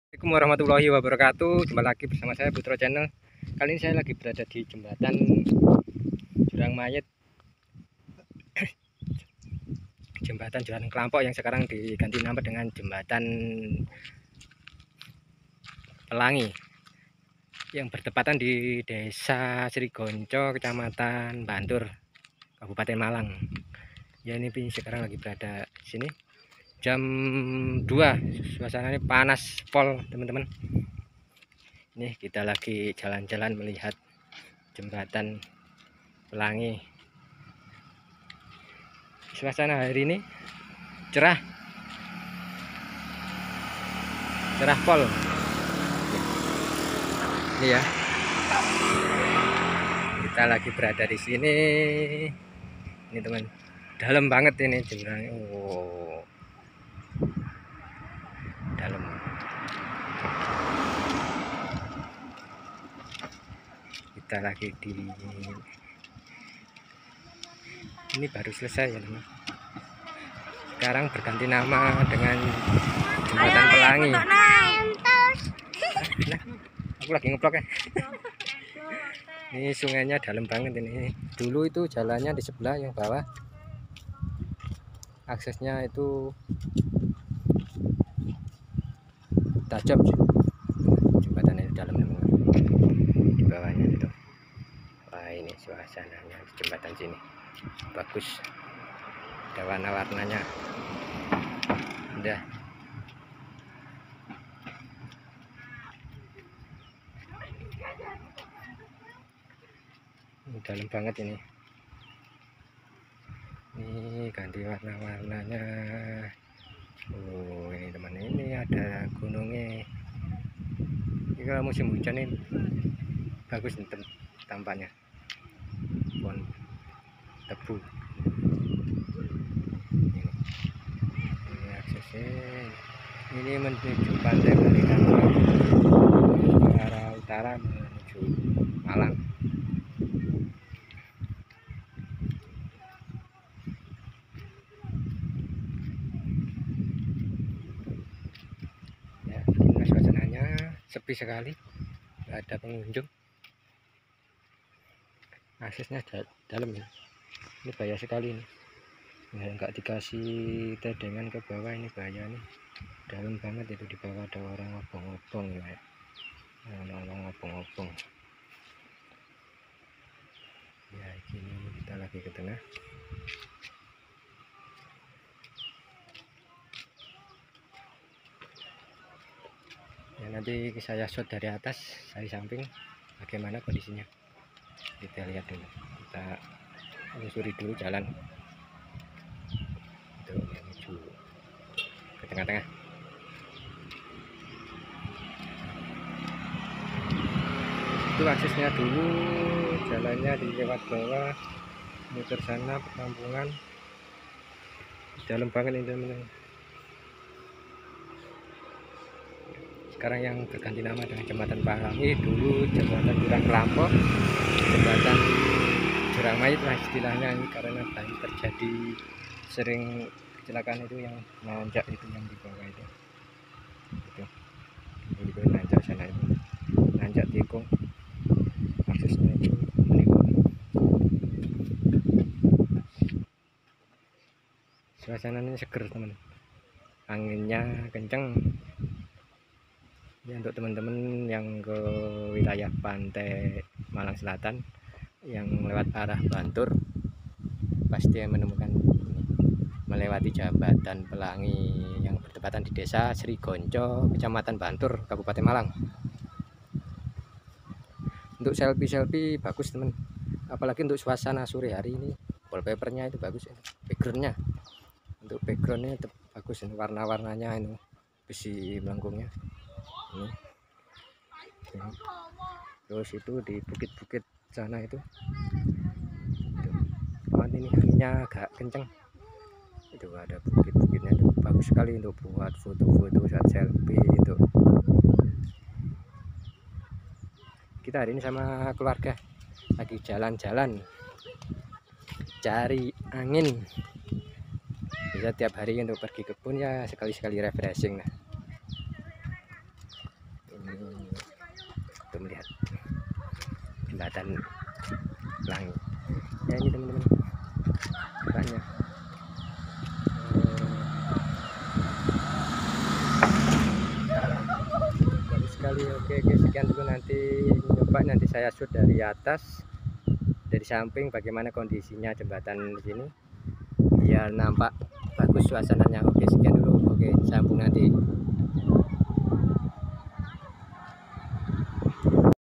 JEMBATAN JURANG MAYIT terbaru menuju wisata pantai selatan#jlsmalang
jembatan jurang Klampok /mayit dan sekarang ganti nama dengan jembatan pelangi terletad desa srigonco kec bantur malang